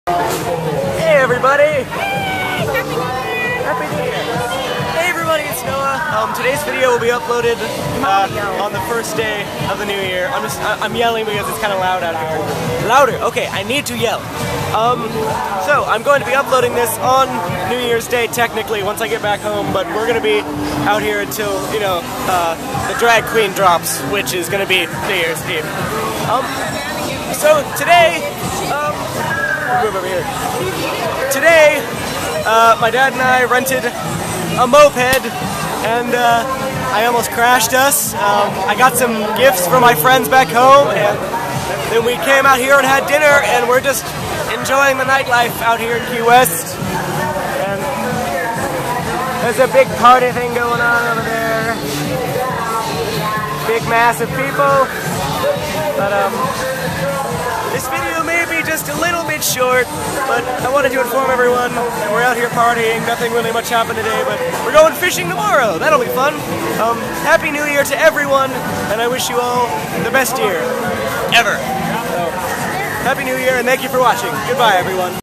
Key West New Years Vlogs day 5: HAPPY NEW YEAR!
New years eve in downtown key west was awesome! A drag queen was dropped. as opposed to a ball, and a bunch of other fun stuff!!!